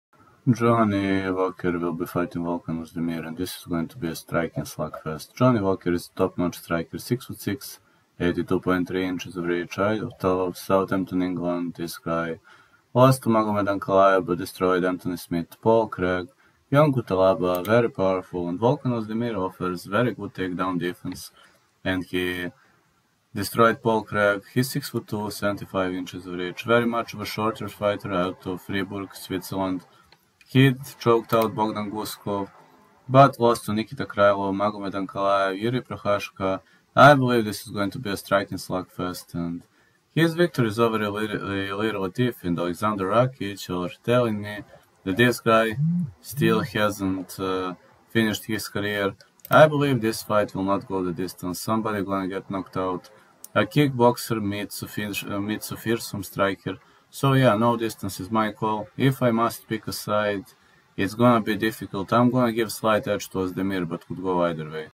Predicting Johnny Walker's Next Fight Against Volkan Oezdemir - Who Will Come Out On Top?
Johnny Walker Vs Volkan Oezdemir - Early Prediction